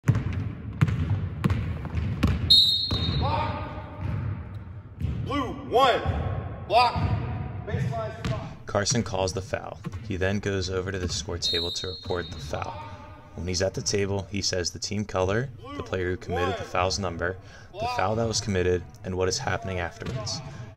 Reporting a Foul to the Table
Carson- Official
Lauren- Table Official
Will- Offense
Spencer- Committed his 27th foul